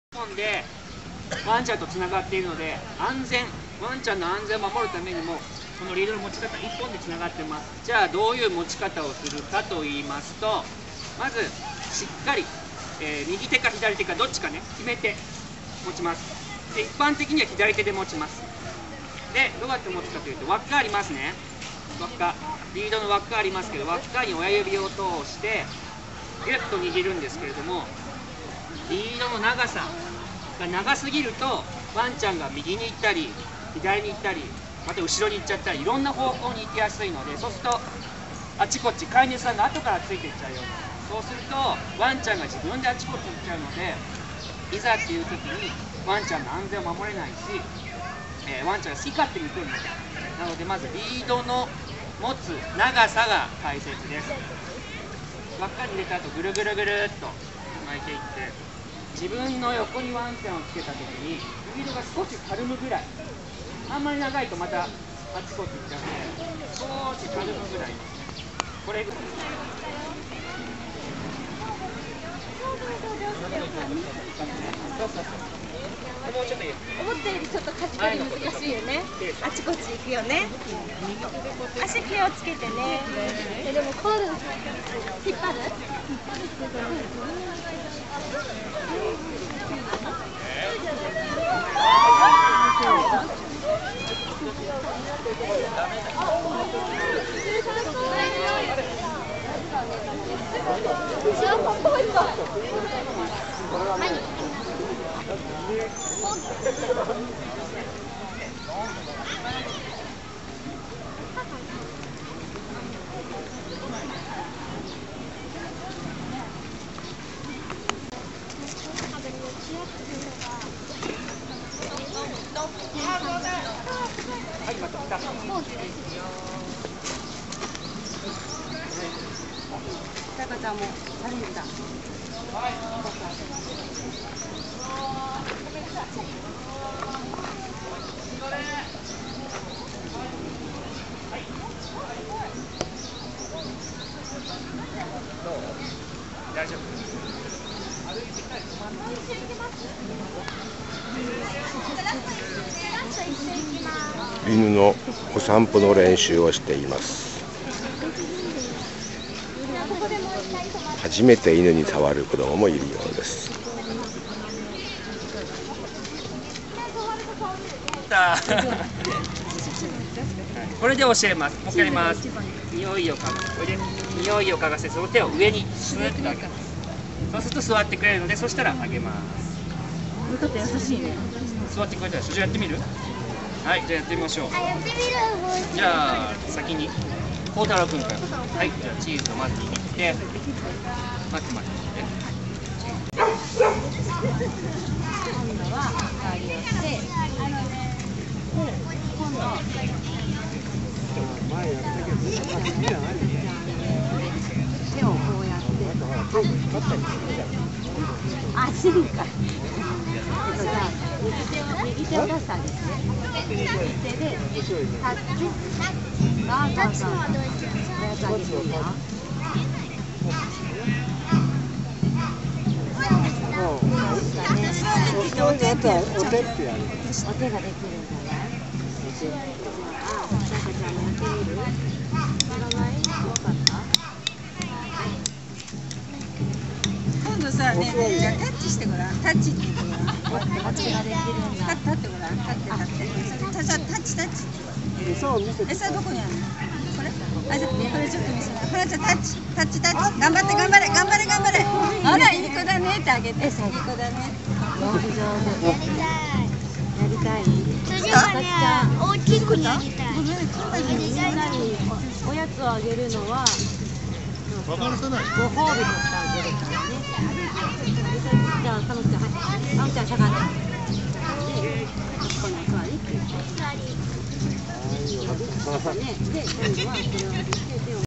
130526「代々木公園で犬と子どもの仲良しトレーニング」
【東京の風景】動物とのつき合いが薄い子ども達のためのドッグトレーニング入門の様子です。基本のコツは「まず自分の臭いをかがせる。顔を見てお腹から触る。耳、尻尾、足は触らない」とのことでした。取材に出かけたおかげで私も近所の犬を触れるようになりました。